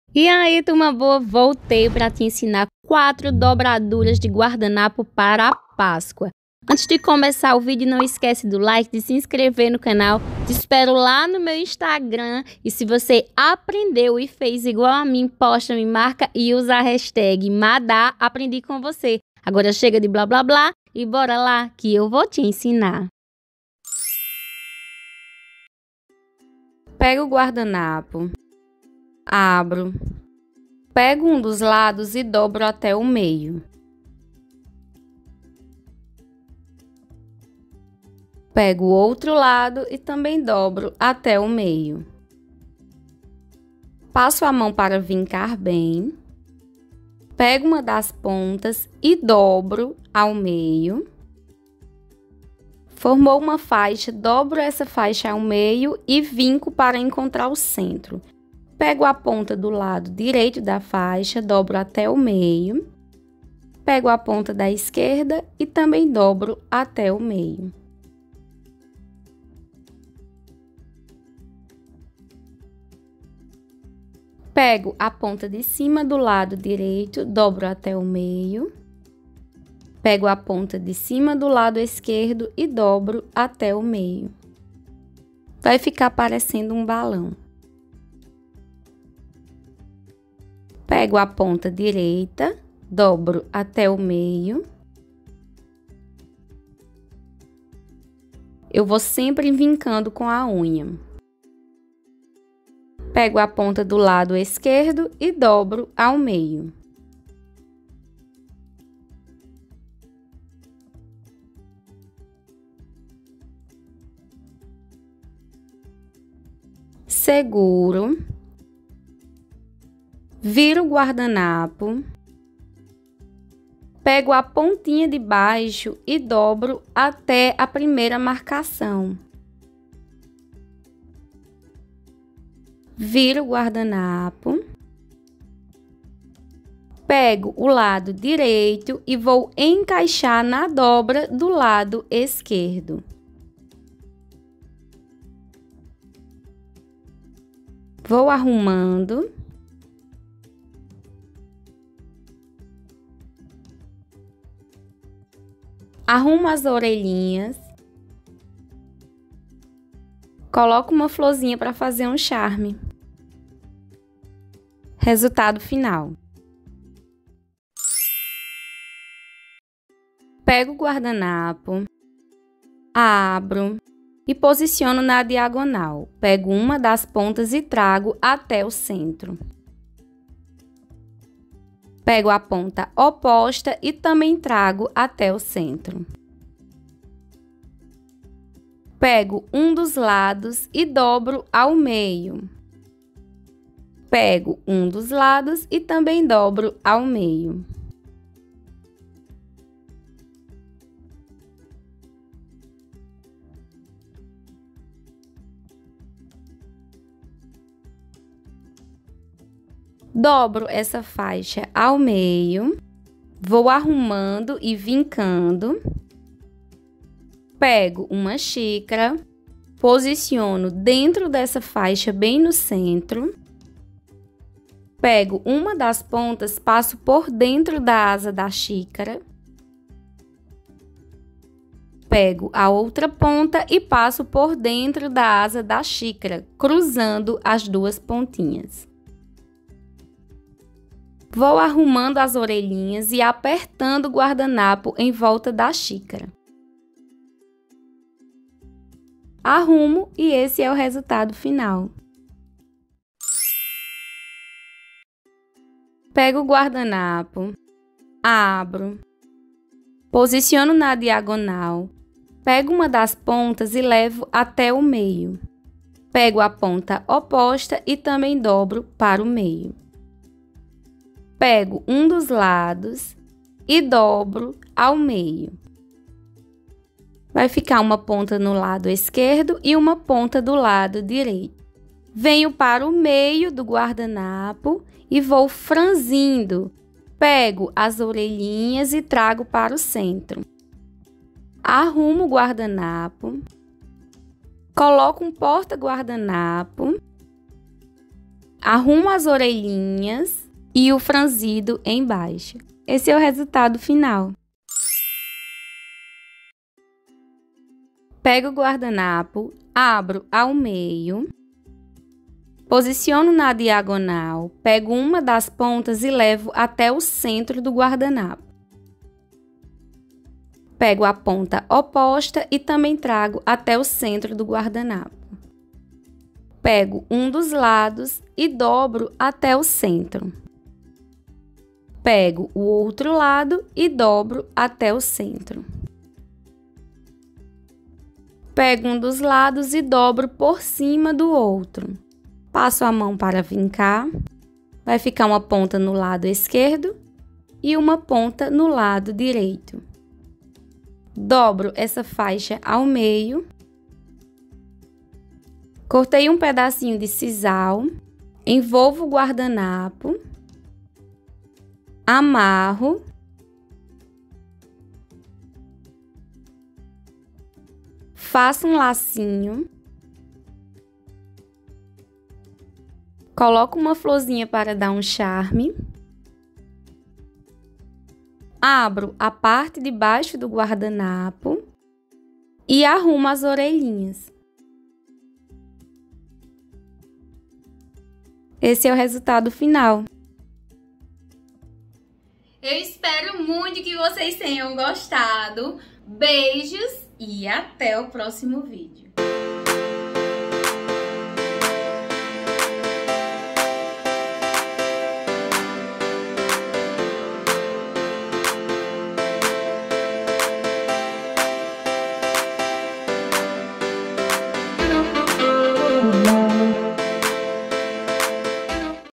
4 Dobraduras de guardanapo para Páscoa
E aí? Turma boa! No vídeo de hoje te ensino 4 dobraduras de guardanapo para Páscoa, espero que gostem! Beijos!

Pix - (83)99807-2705

📸 Fotos -